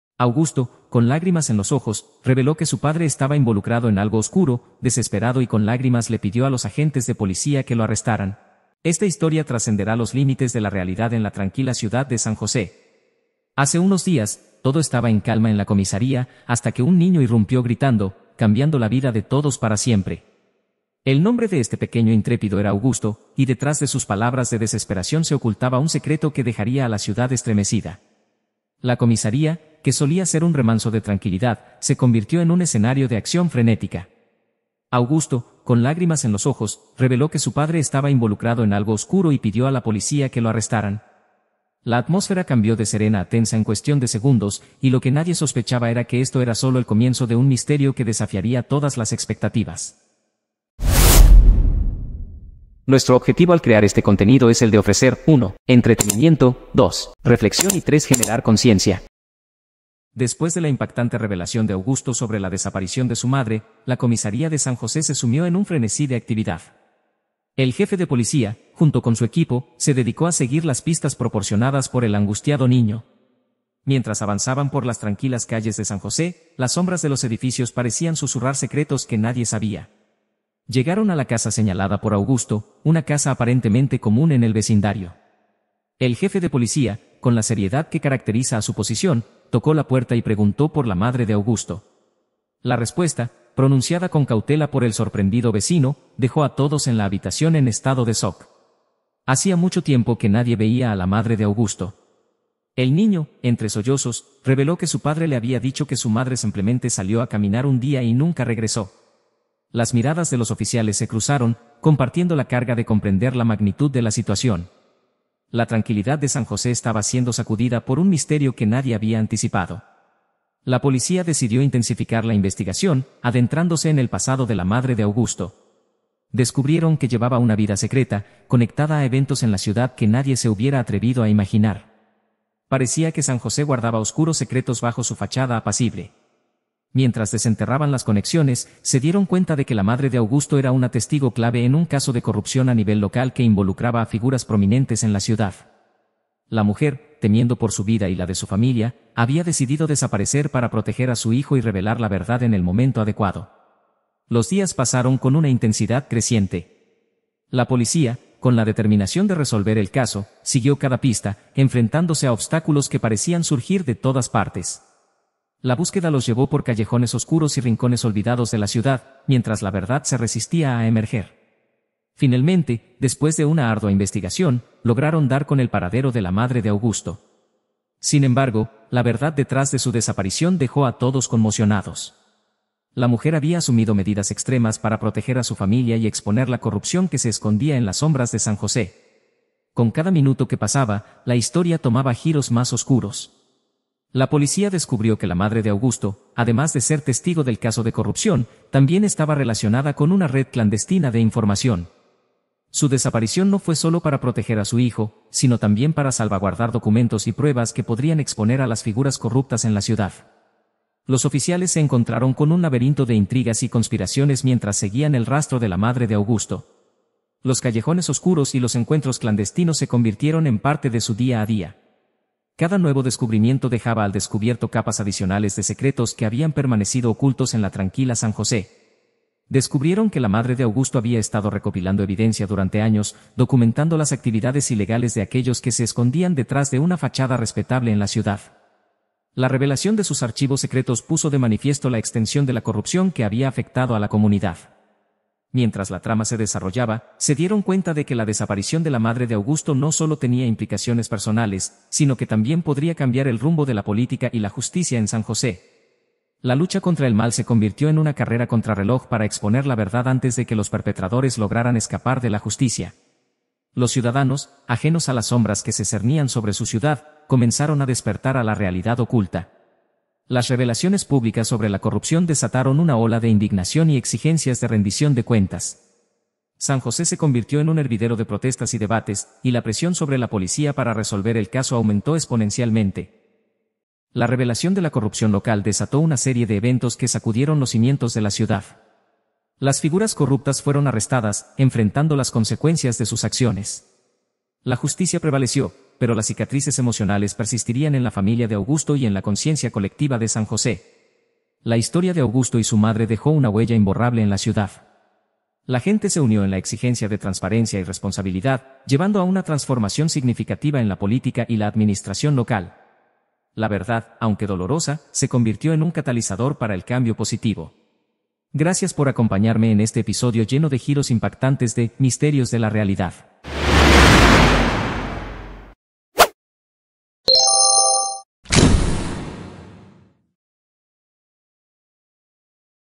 🔴 El niño, con Lágrimas en los Ojos, pide a POLICIAS ARRESTAR su PARDRE
Augusto, con lágrimas en los ojos, reveló que su padre estaba involucrado en algo oscuro, desesperado y con lagrimas le pidió a los agentes de  policía que lo arrestaran.